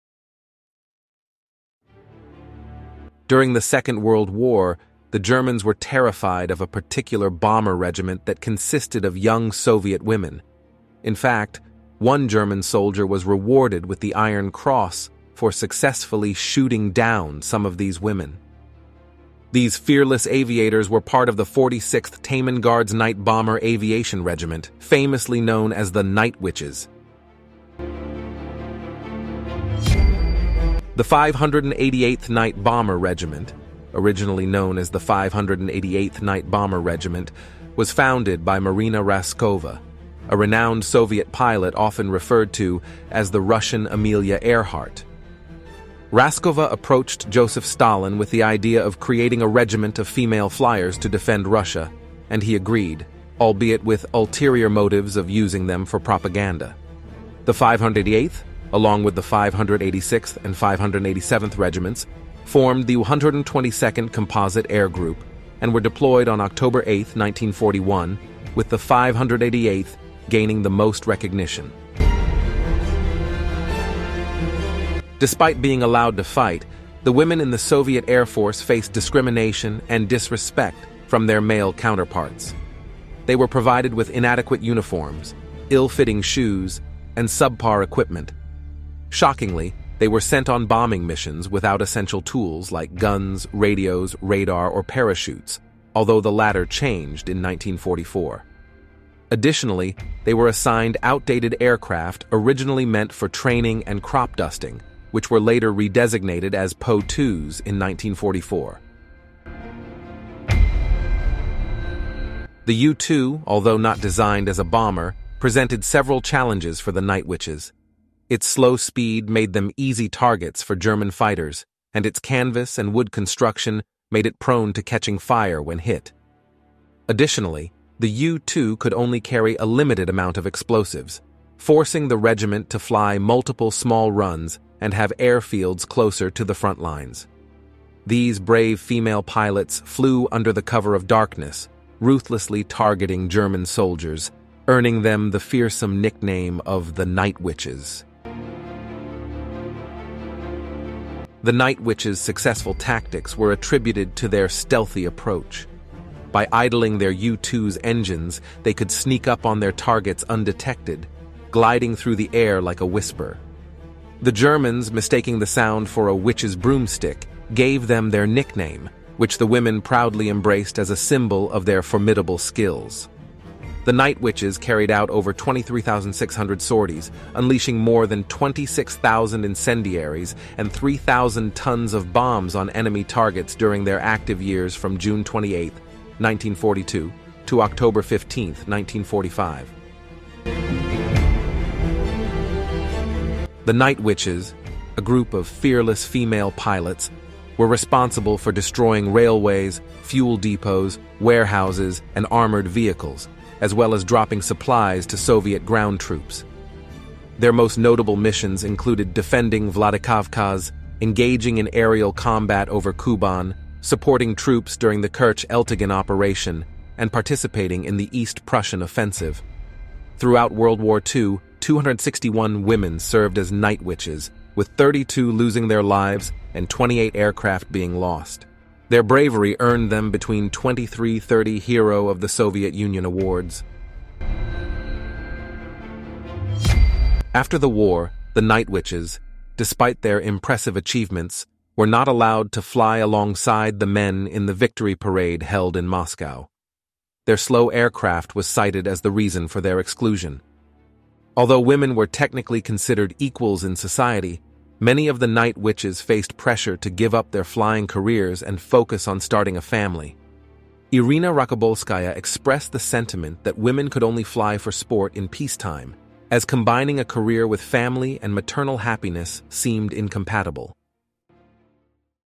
The Fearless Night Bombers of the 46th Taman Guards Regiment
During the Second World War, a remarkable group of young Soviet women in their late teens and early 20s formed the 46th Taman Guards Night Bomber Aviation Regiment. These brave individuals, known as the 'Night Witches,' struck fear into the hearts of German soldiers. This article explores the incredible story of these fearless night bombers who defied gender norms and bombed German soldiers with precision and determination.